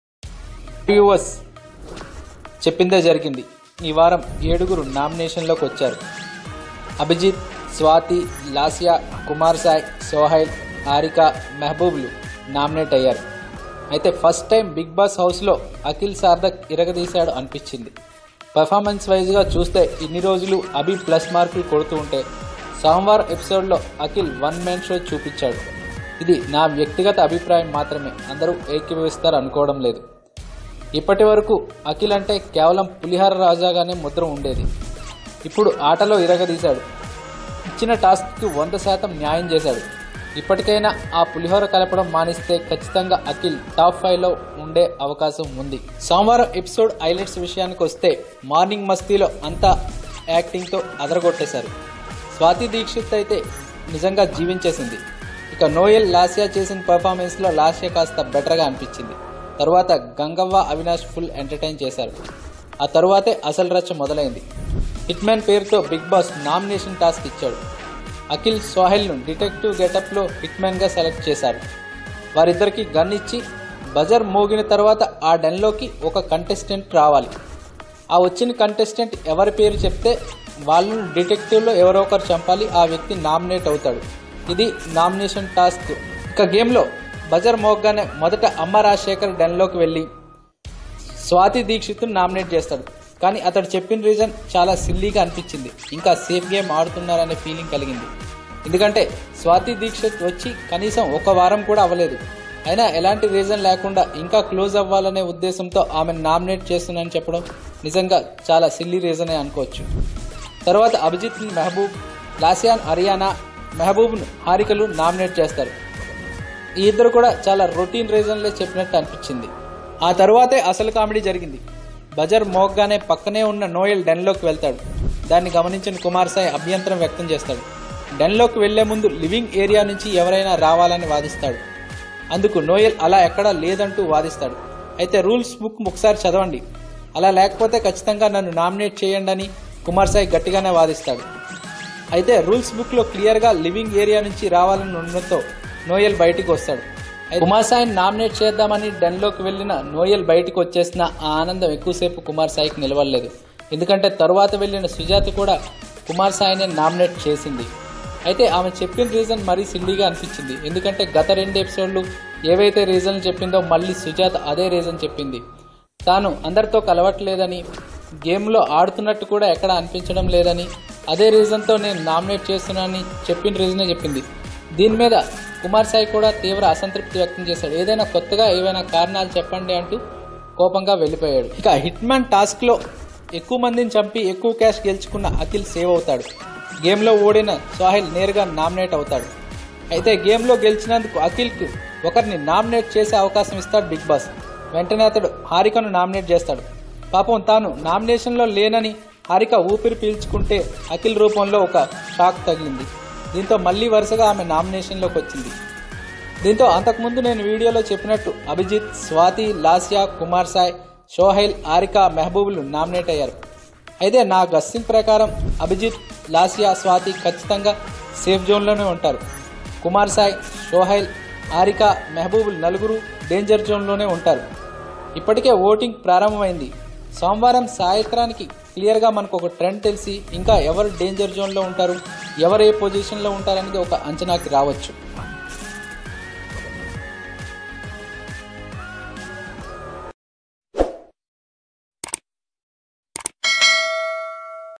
First time akhil one man show in BB house
అభిజిత్, స్వాతి, లాస్య, కుమార్ సాయి, సోహైల్, హారిక, మెహబూబ్ లు నామినేట్ అయ్యారు. ఫస్ట్ టైం బిగ్ బాస్ హౌస్  లో అఖిల్ సార్ధక్ ఇరగదీశాడు అనిపించింది. పర్ఫార్మెన్స్ వైజ్ గా ఇన్ని రోజులు అభి ప్లస్ మార్కులు కొడుతుంటే.. సోమవారం ఎపిసోడ్ లో అఖిల్ వన్ మెన్ షో అనిపిచింది. ఇది నా వ్యాక్తిగత అభిప్రాయం మాత్రమే అందరూ ఏకీభవిస్తారు అనుకోవడం లేదు. ఇప్పటి వరకు అఖిల్ అంటే కేవలం పులిహోర రాజాగానే ముద్ర ఉండేది.. ఇప్పుడు ఆటలో ఇరగదీశాడు.. టాస్క్ కు వందకు వంద శాతం న్యాయం చేశాడు.. ఇప్పటికైనా ఆ పులిహోర కలపడం మానేస్తే కచ్చితంగా టాఫ్ ఫైవ్ లో ఉండే అవకాశం లేకపోలేదు..